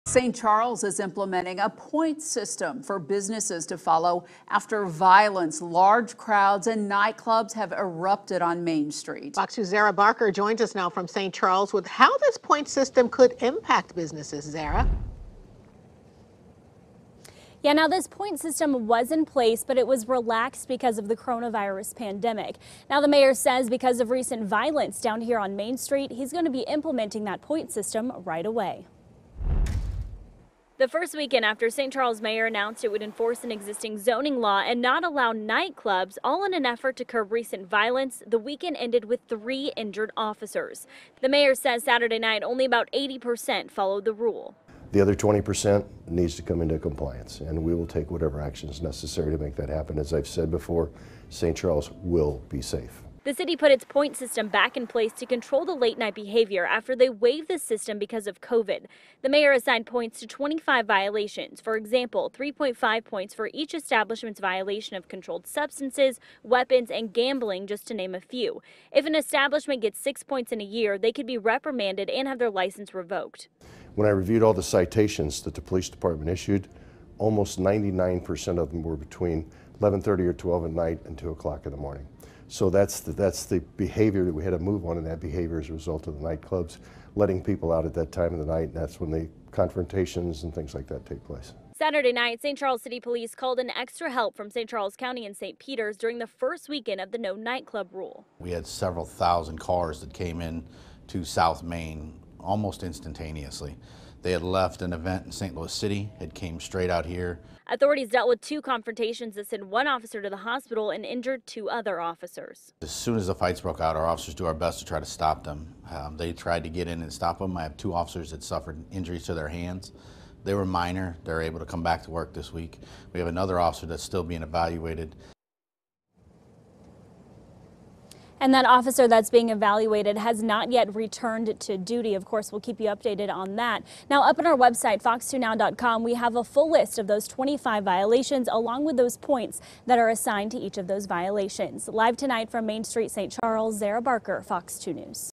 Point system now in effect for Main Street businesses after officers injured during weekend violence
St. Charles City Mayor Dan Borgmeyer said the city is putting its point system back into place to try and control the late-night behavior that has turned violent on some nights. The point system was in place but relaxed due to the coronavirus.